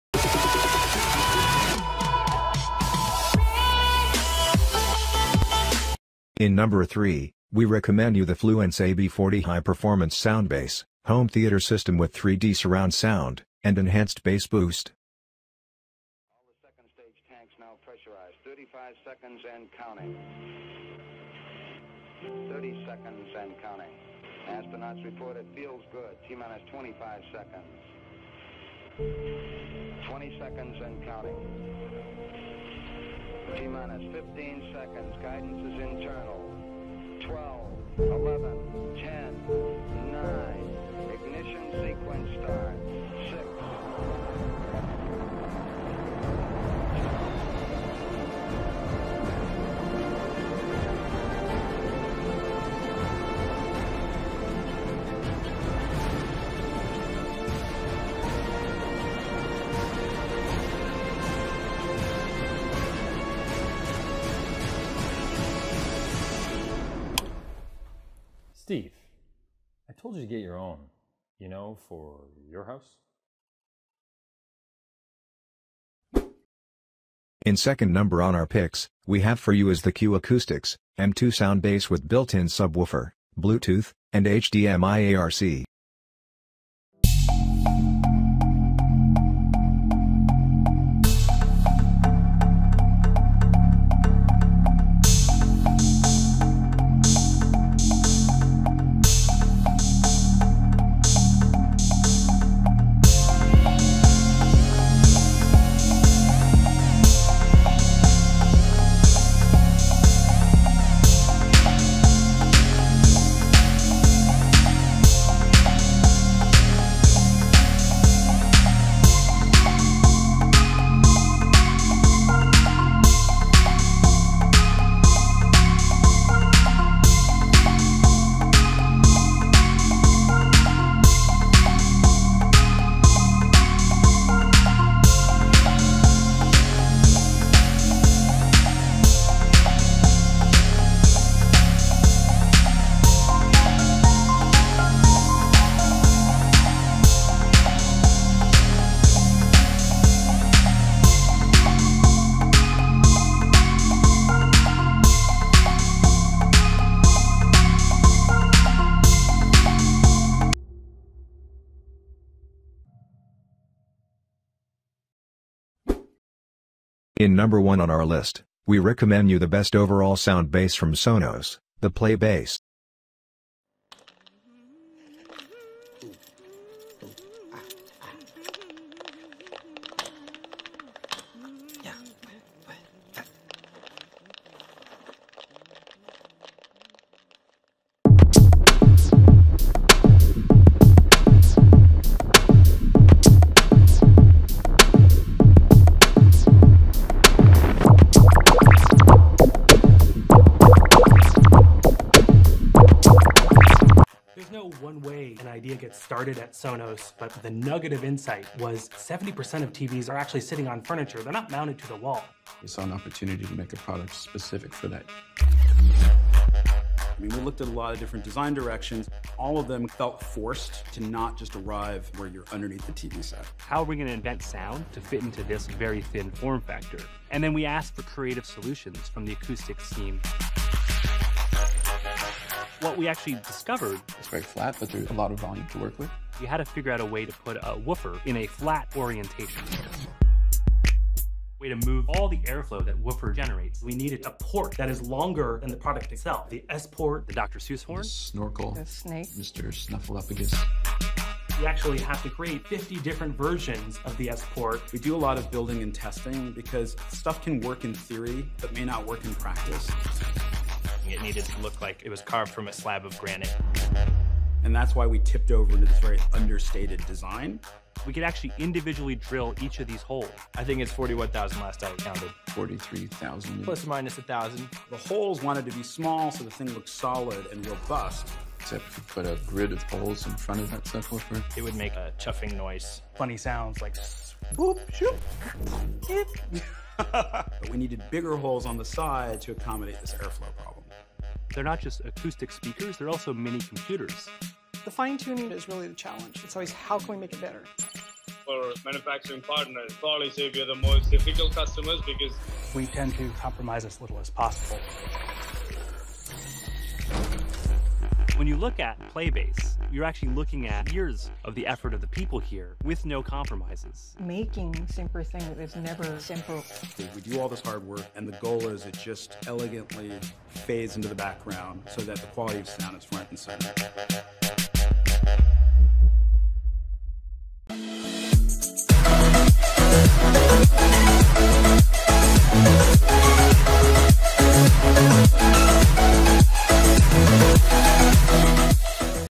Best Soundbar 2019|Essential Gadgets
product links::

3. Fluance AB40 High-Performance Soundbase
2. Q Acoustics M2 Soundbase with Built-in Subwoofer
1. Sonos Playbase – For Widescreen Sound and Music Streaming